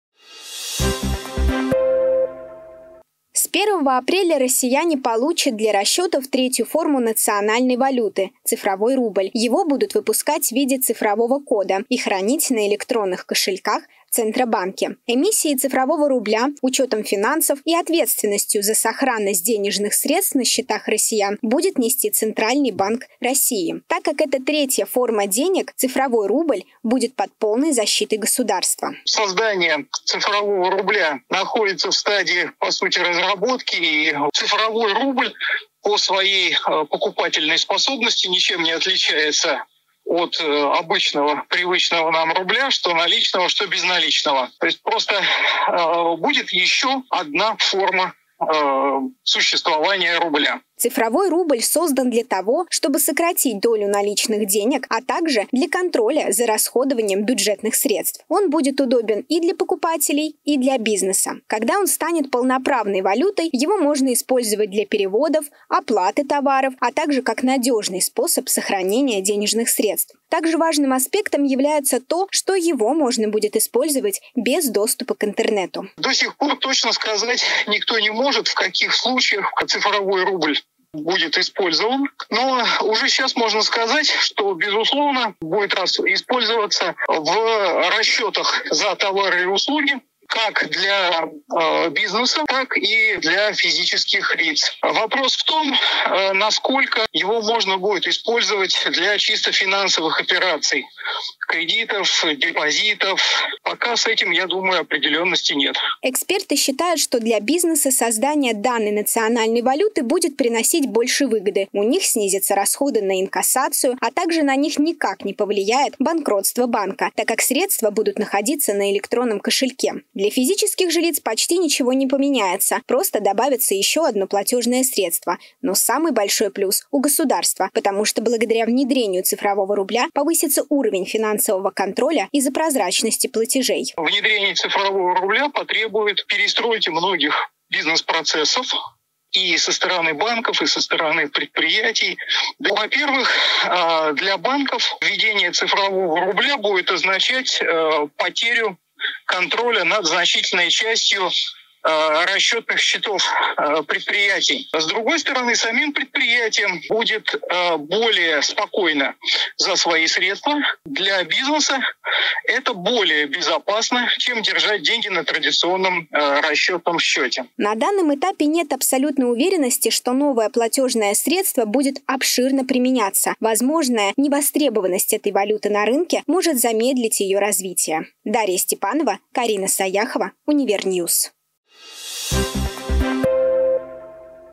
ЦИФРОВАЯ КОПЕЙКА БЕРЕЖЕТ ЦИФРОВОЙ РУБЛЬ
Госдума одобрила введение цифрового рубля. В скором времени в России наряду с наличными и безналичными способами оплаты появится третья форма национальной валюты.